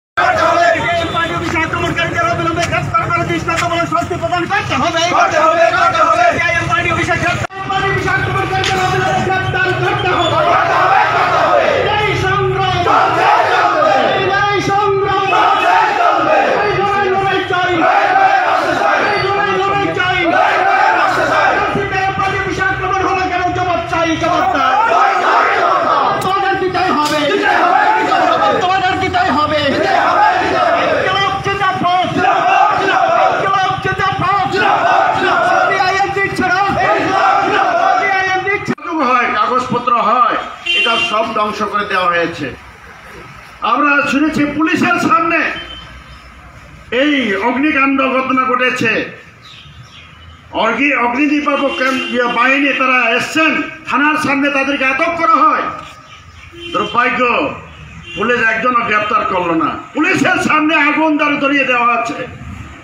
সন্ত্রাসীদের দৃষ্টান্তমূলক শাস্তির দাবিতে গর্জে উঠলো গন্ডাছড়া
Video from DIBYENDU DEY